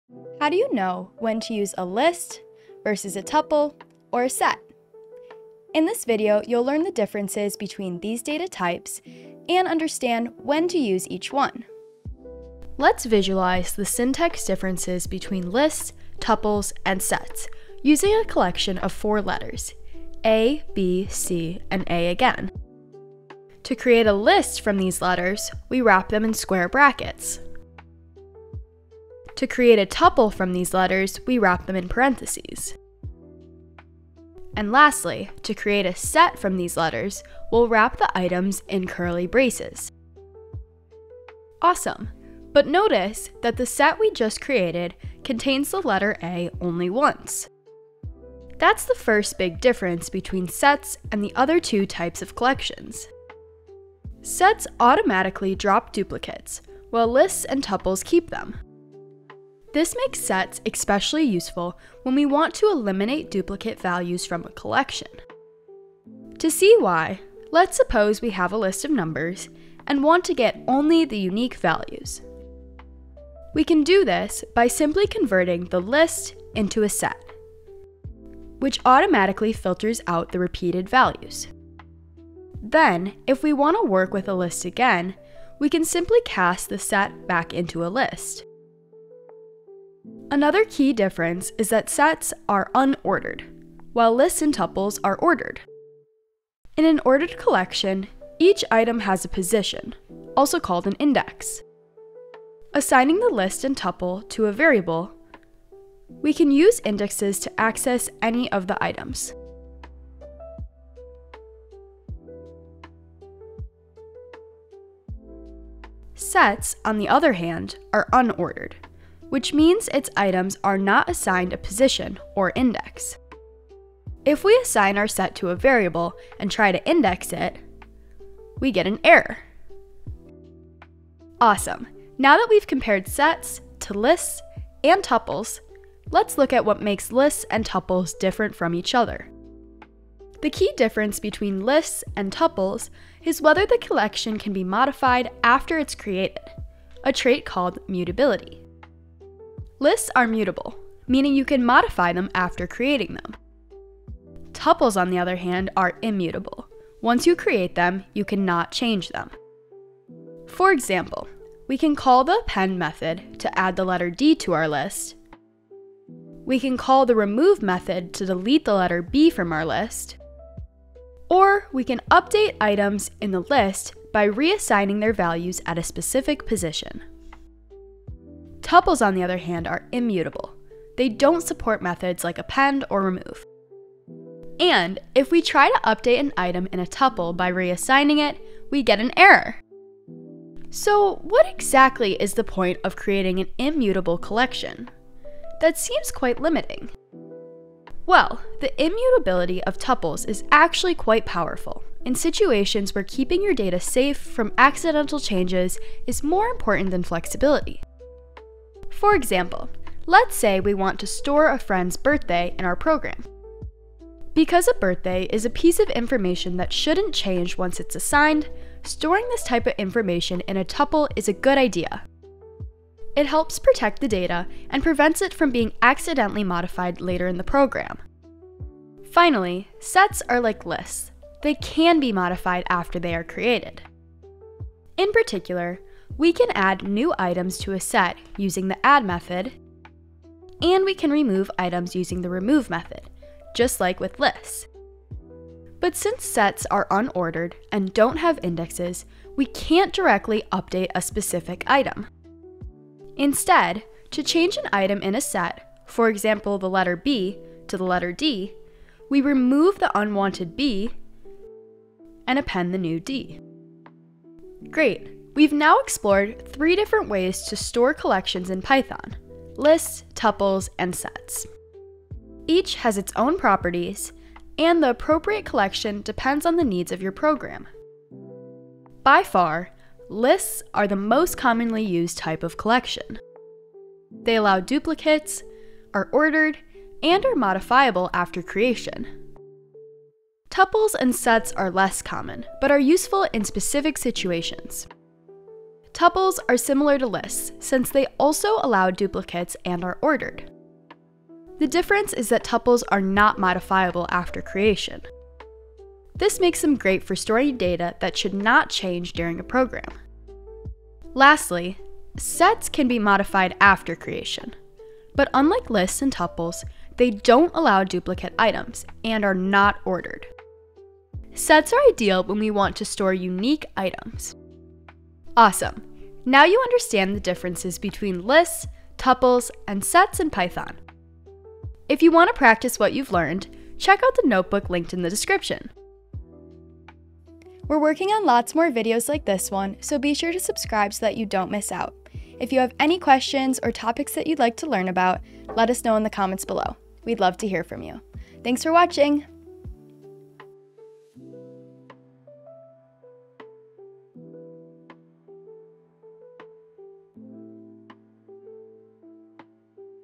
Python Lists vs Tuples vs Sets - Visually Explained
🔵 Resources & Further Learning

🔵 Chapters
00:00 - Intro
00:15 - Syntax differences
00:52 - Sets vs lists & tuples
02:19 - Lists vs tuples
04:27 - Summary

🔵 Python Lists vs Tuples vs Sets
In this video, we explore the key differences between Python's lists, tuples, and sets. We also guide you through the specific scenarios where each data structure shines.